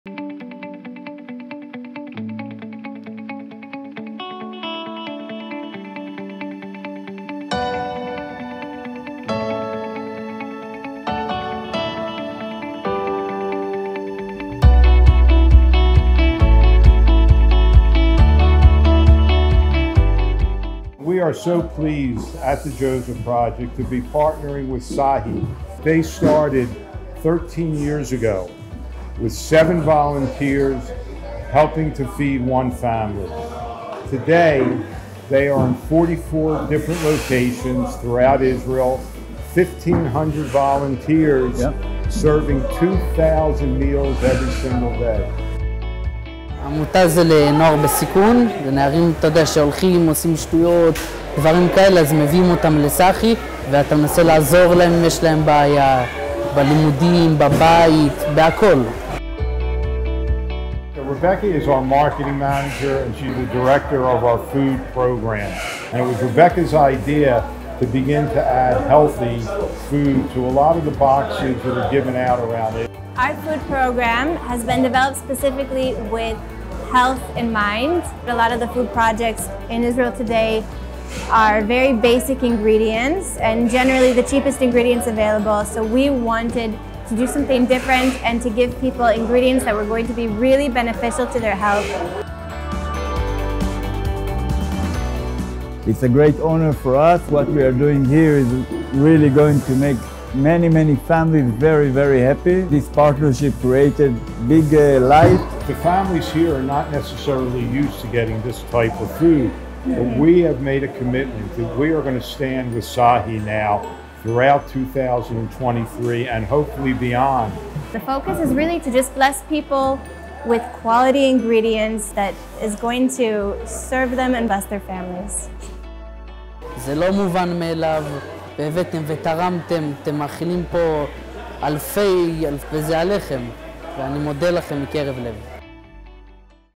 Partnership with SAHI to Distribute 1,000 Boxes of Food
Partnering with @SahiIsrael to bring 1,000 boxes of food to families in need throughout Jerusalem.